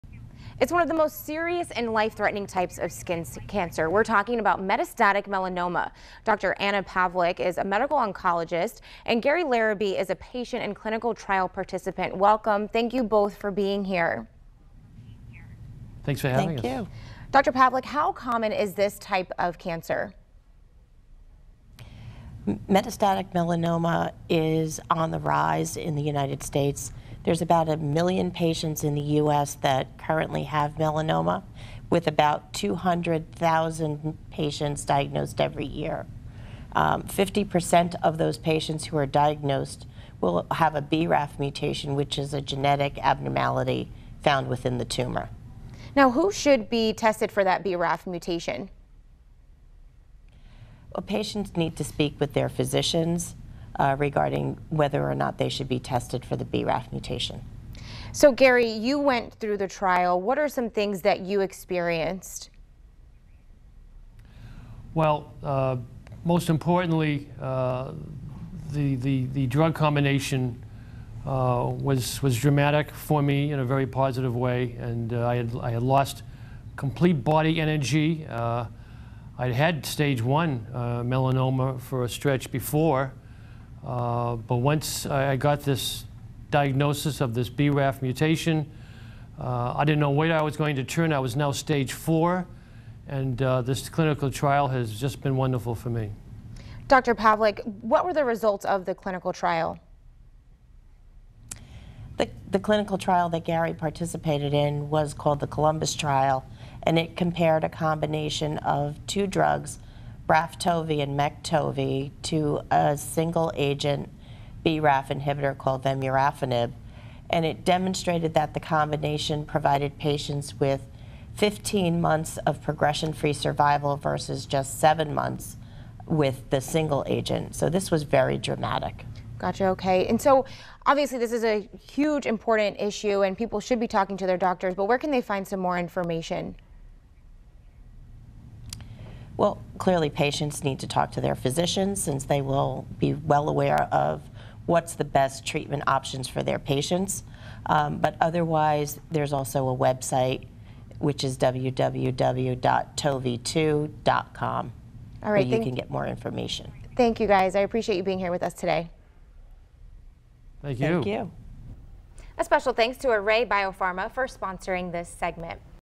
FCL Friday December 14th Symptoms of Metastatic Melanoma
Metastatic Melanoma is one of the most serious and life-threatening types of skin cancer.